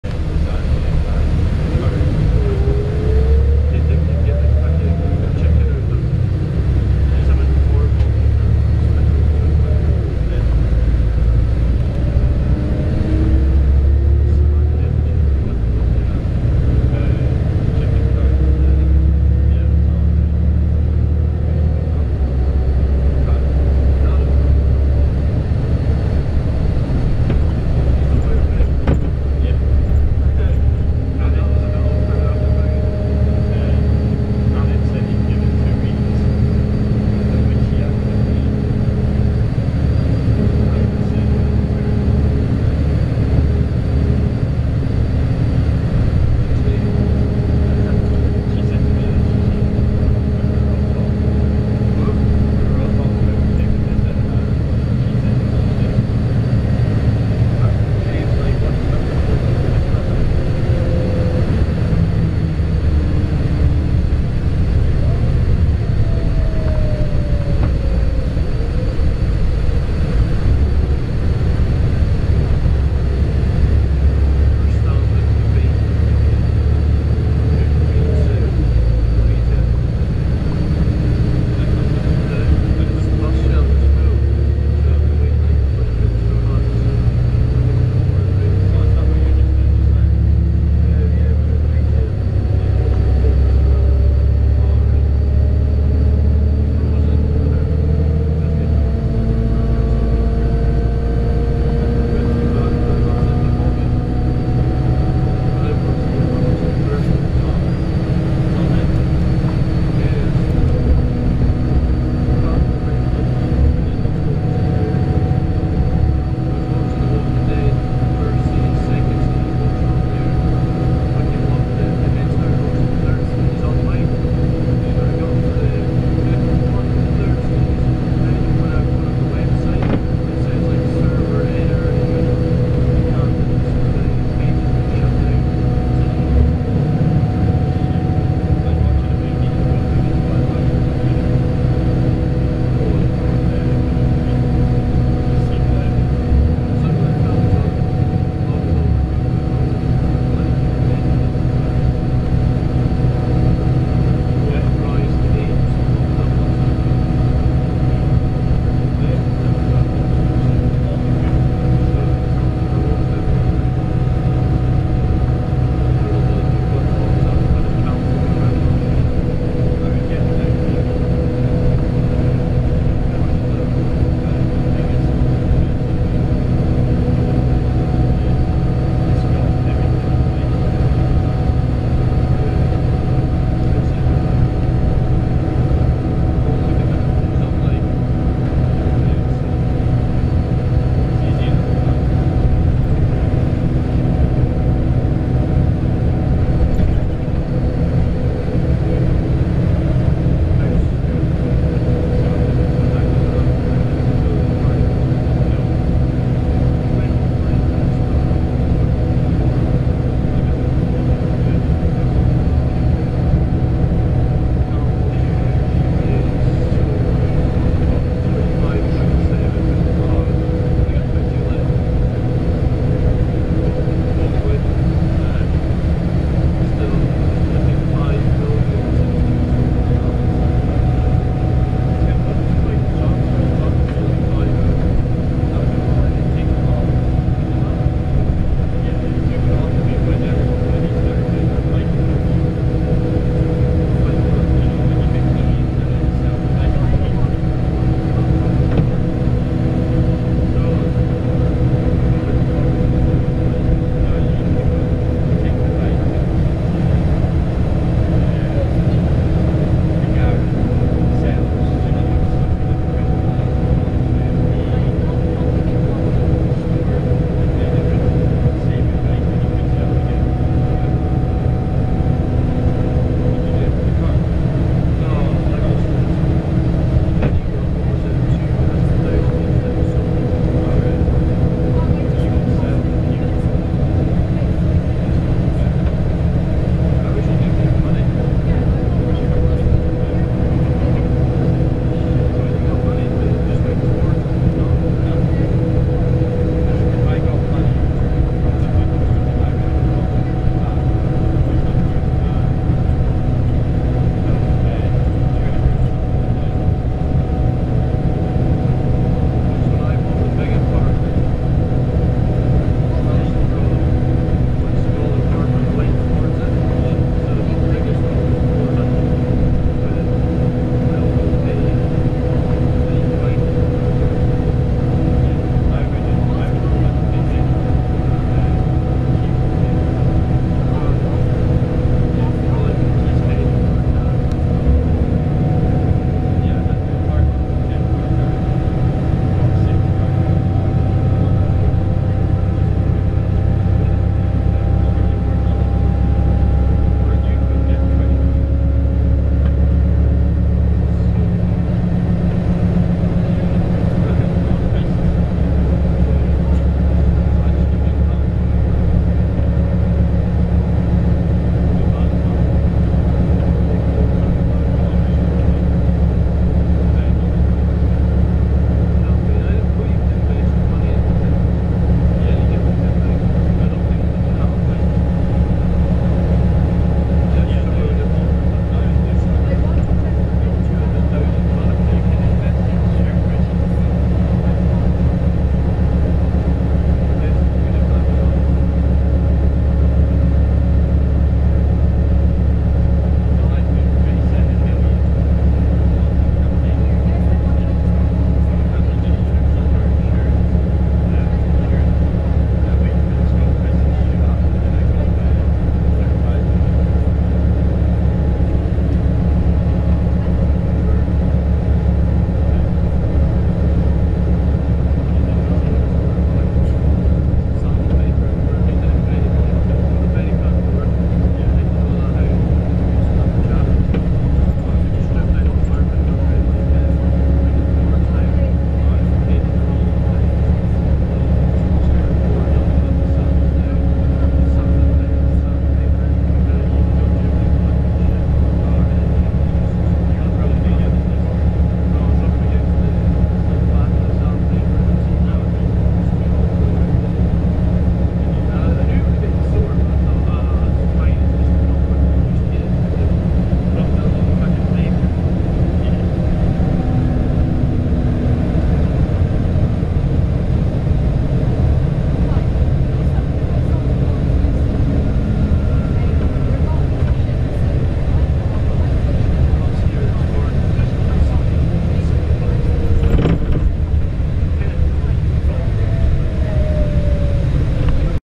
Route 10B Stagecoach Bluebird Volvo B7R Plaxton Profile 53242 (SV55 FJU)
Operator: Stagecoach Bluebird
Depot Based: Elgin
Engine Chassis: Volvo B7R
Bus Bodywork: Plaxton Profile
Fleet Number: 53242
Registration Plate: (SV55 FJU)
Route Service: 10B